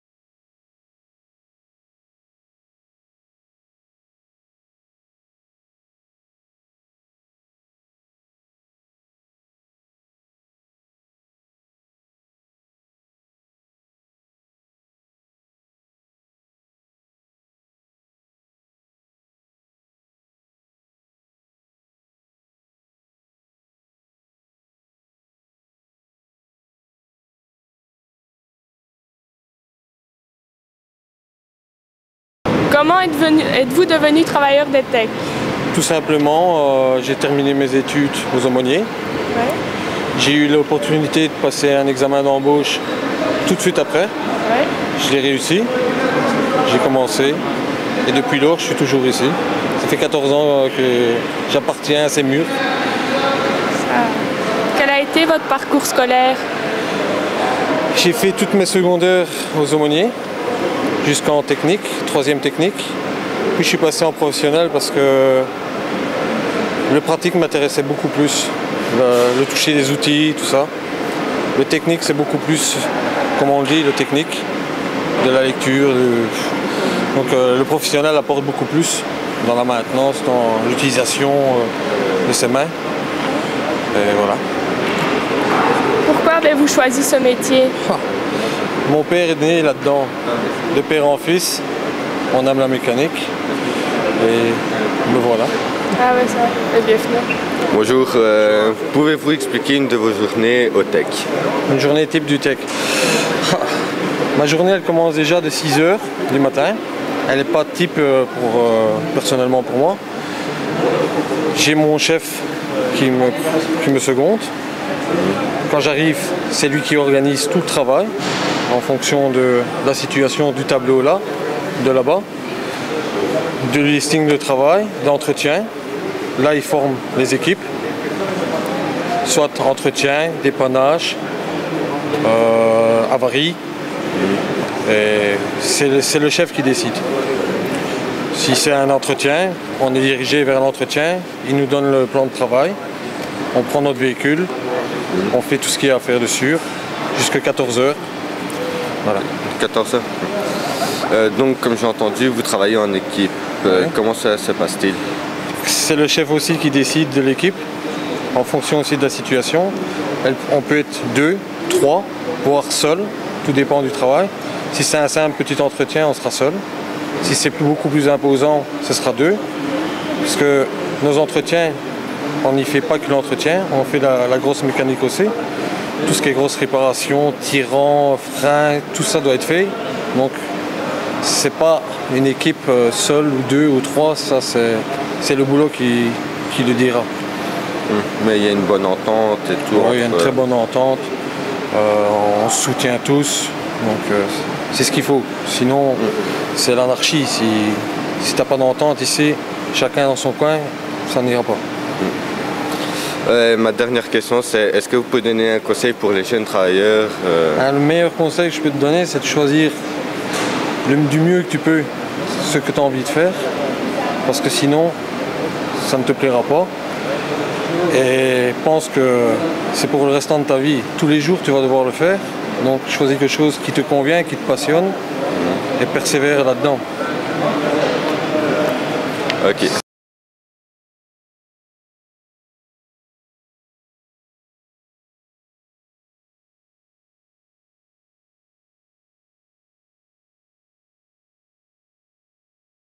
Interview de M. Lebrun, Mécanicien d'Entretien TEC Charleroi
Les métiers du TEC Charleroi. Une organisation Bugpixel et Ordinoado avec le soutien du Fonds Mestdagh, pour une société pleine d'opportunités, géré par la Fondation Roi Baudouin.
Interview réalisé par la classe 3P MP (Aumôniers du Travail Charleroi).